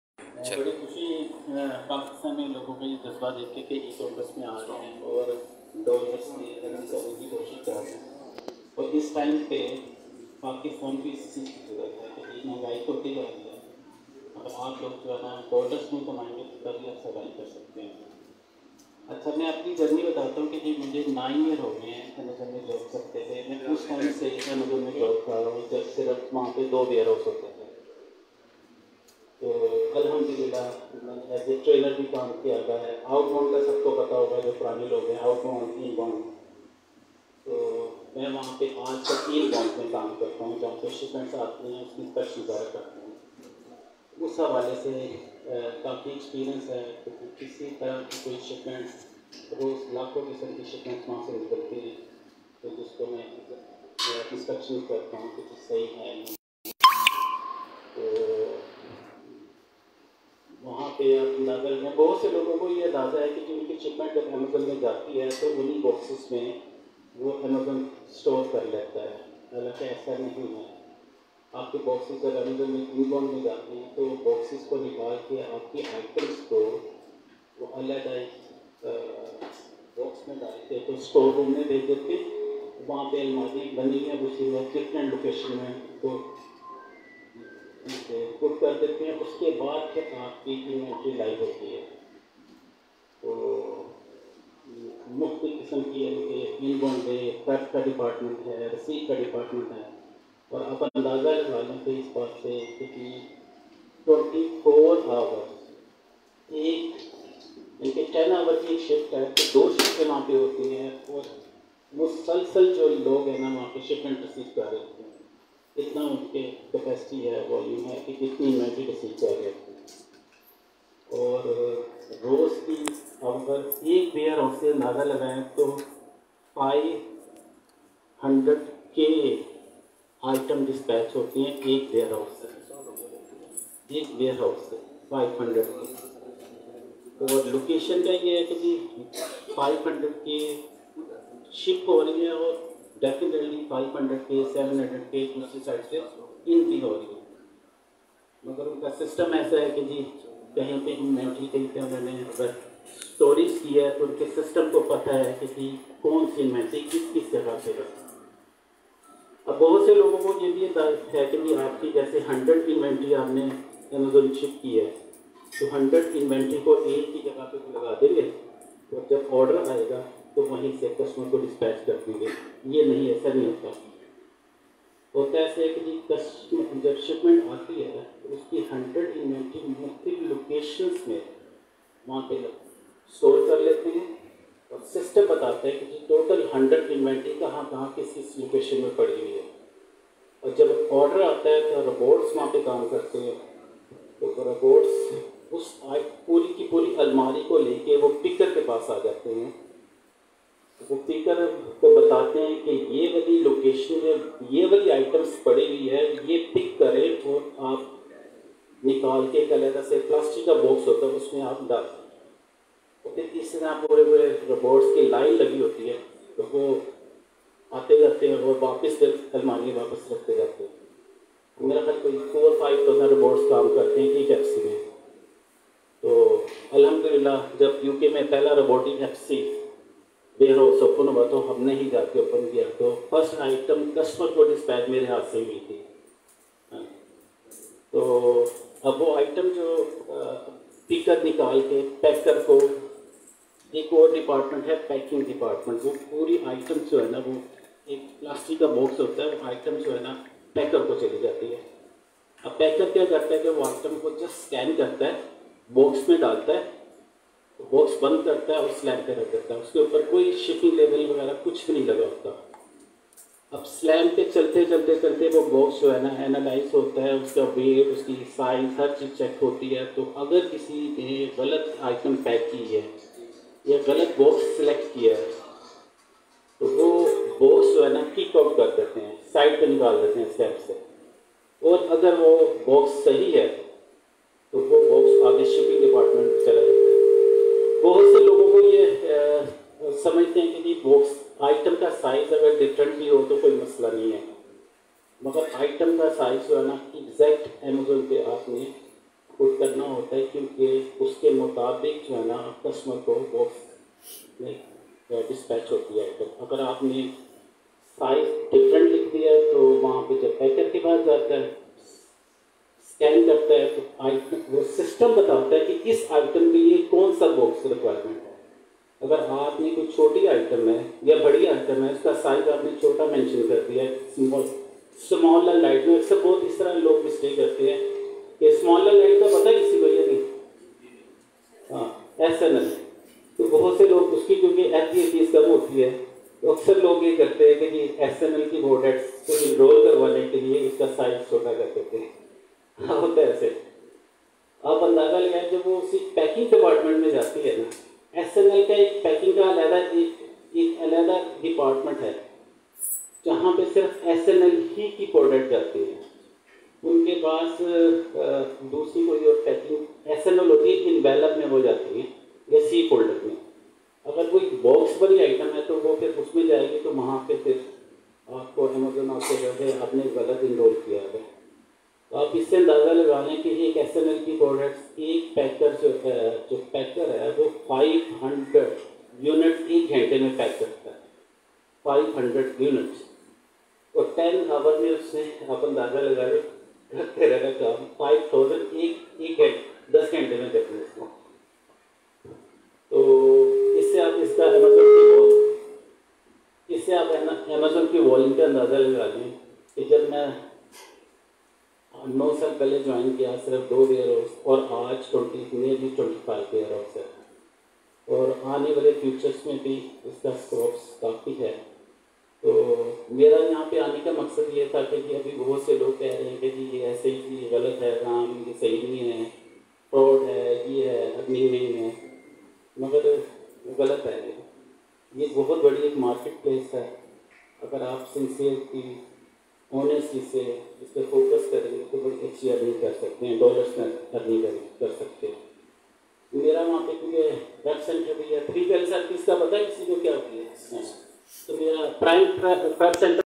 How Amazon Works? | Syed Humayun Saleem - Director PimePrep Center & Amazon Employee | Live Session
Syed Humayun Saleem - Director PimePrep Center & Amazon Employee

Live session in WORC lahore
Host: *Syed Humayun Saleem*(Director PimePrep Center and Currently doing job in Amazon)
Topic: *Learn about the inner workings of Amazon*